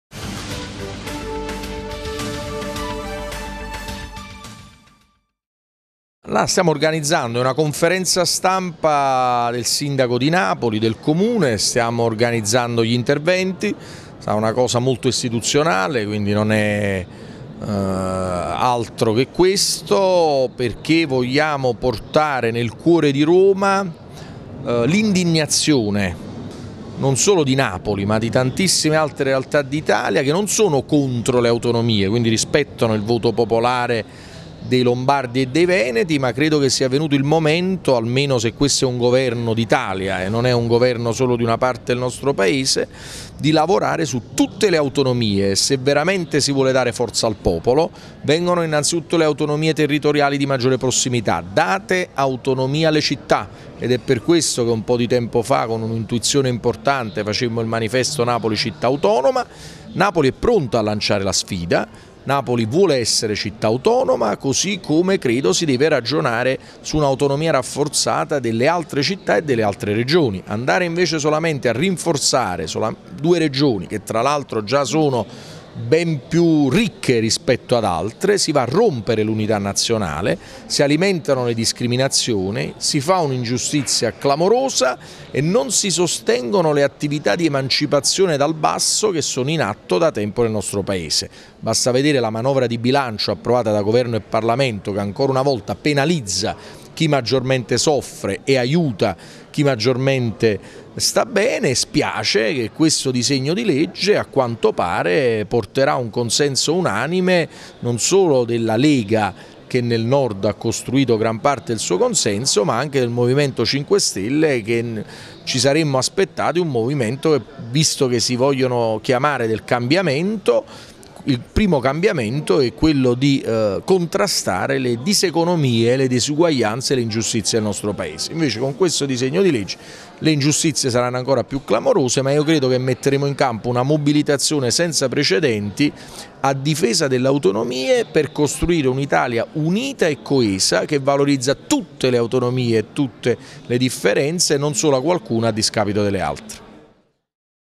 De Magistris: in piazza Montecitorio porteremo l'indignazione di Napoli
Il sindaco di Napoli Luigi de Magistris annuncia un'iniziativa in piazza Montecitorio a Roma in favore delle autonomie cittadine.